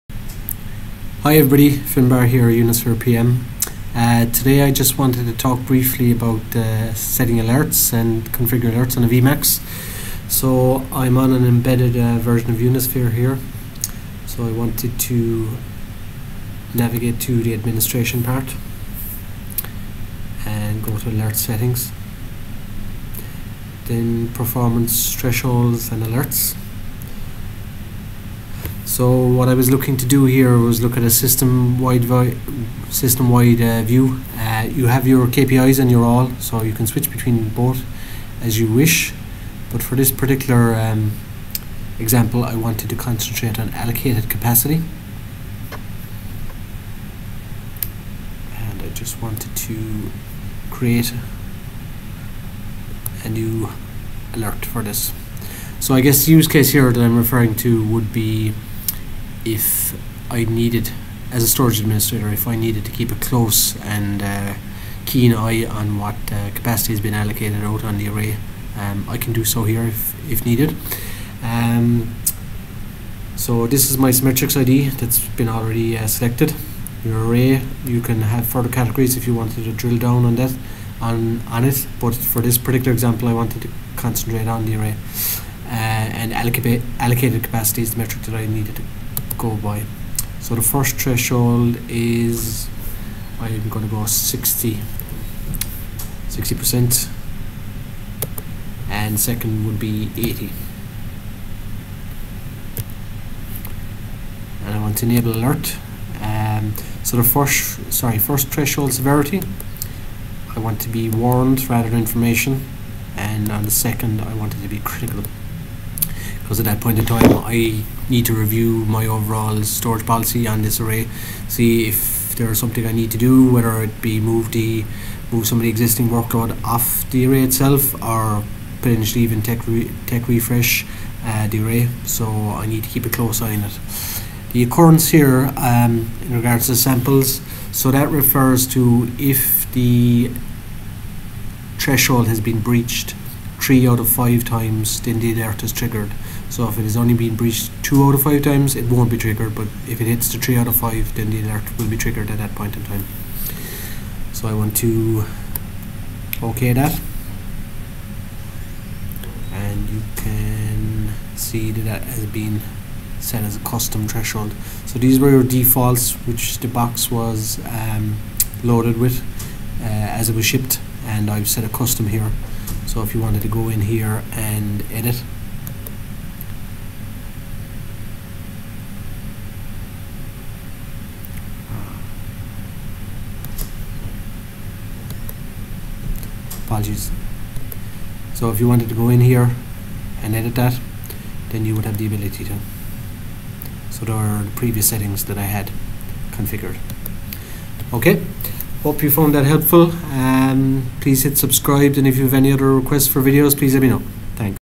VMAX Alerts Configuring and editing your alert Tresholds
This video shows you how to configure your alerts in Unisphere and in this example we focus on allocated capacity.